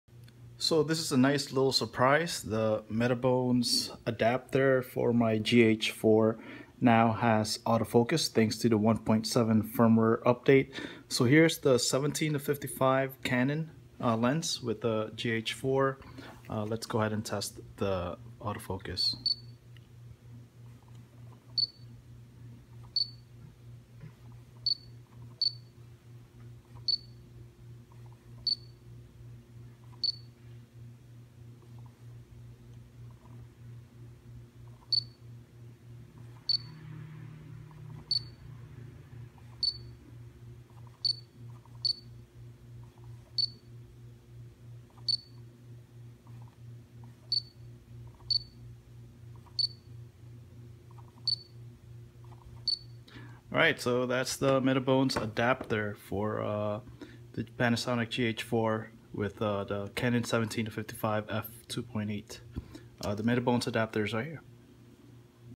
Metabones speed booster Canon to Micro Four thirds with Autofocus
Metabones released a firmware update, version 1.7, to their speed booster adapter that adds autofocus to Canon lens. Previously, I had to do manual focus. This video shows the Canon 17-55mm F/2.8 lens attached to the Panasonic GH4 using the Metabones Speed Booster adapter.